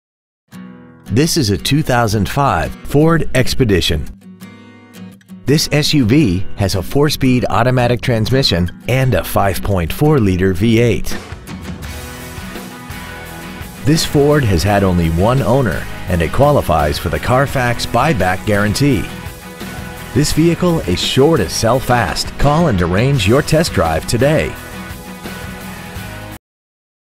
Pre-Owned 2005 Ford Expedition Houston TX 77027
Year : 2005
 Make : Ford
 Model : Expedition
 Engine : Gas V8 5.4L/330
 Trans . : Automatic
 Exterior : Black
 Miles : 69,659
 Interior : TAN